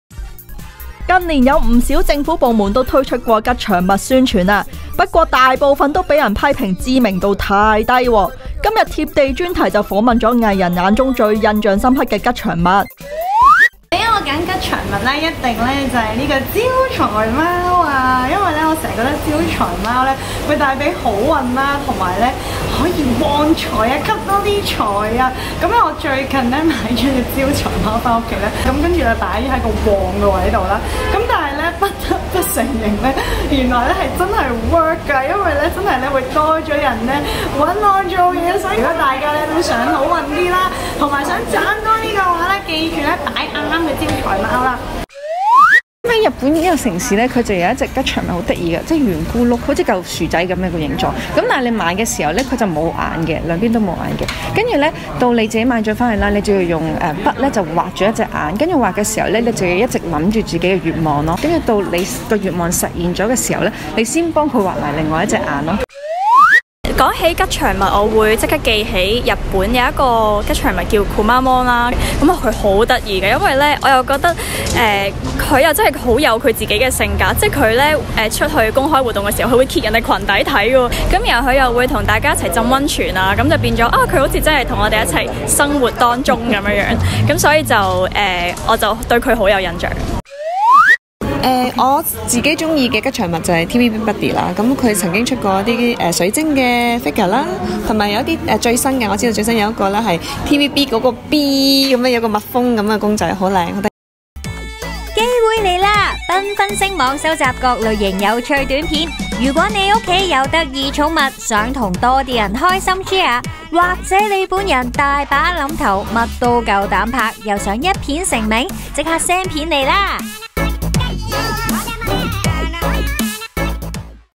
吉祥物比併 蘇麗珊熊本熊戰楊卓娜TV Buddy
近年有不少政府部門都會推出吉祥物宣傳，不過大部分都被批評知名度低，今日貼地專題就訪問藝人眼中最深的吉祥物，為何有吸引之處？
蘇麗珊同熊本熊一齊浸溫泉。
蘇麗珊對熊本熊好有親切感。
女神蘇麗珊：「記起日本有隻吉祥物叫熊本熊（Kumamon），係一隻黑色熊嚟嘅，好得意，佢好有自己性格，會喺活動揭人裙底，好百厭，又會一齊浸溫泉，好似同我哋一齊生活當中咁，所以對佢好有印象。」
簡淑兒為達摩不倒翁開眼願望達成。
好多時達摩不倒翁剛買回來時是沒有眼睛的，加了眼睛許願，會好快事成。
「少女標本」成員簡淑兒：「日本有隻吉祥物叫達摩不倒翁，圓咕碌，個形狀好似薯仔咁，你買嘅時候佢係冇眼，你買咗返屋企之後要幫佢畫一隻眼，一邊畫要一邊諗住自己嘅願望，跟住你放喺屋企，到你實現咗個願望之後，你先可以畫埋另一隻眼。我屋企有幾隻喺單眼，因為我啲願望全部未實現，我買咗幾隻，因為有分愛情、事業、生活等等，唔同願望有唔同顏色。」
楊卓娜見到TV Buddy嘅擺設就會買。
楊卓娜對於TVB嘅Bee亦情有獨鍾。
藝人楊卓娜：「我最鍾意嘅吉祥物係TV Buddy，佢曾經出過水晶Figure同最近有TVB嘅Bee，蜜蜂公仔都好靚，我唔算係收藏好多，但見到得意就會買。」
阮兒大讚招財貓為她帶來好運。
荷蘭籍女藝人阮兒：「畀我揀吉祥物，我就會揀招財貓，因為會帶嚟好運，可以旺財，最近我買咗隻招財貓，有成10吋咁大，放咗個旺位，真係不得不承認，原來真係Work，真係多咗人搵我做嘢，真係好好呀！大家都想好運啲、想賺多啲，就要放番隻招財貓啦！」
「繽FUN星網」誠招各類有趣短片！
家有可愛貓狗，想同更多嘅人分享？
有橋有料，素人想一片成名？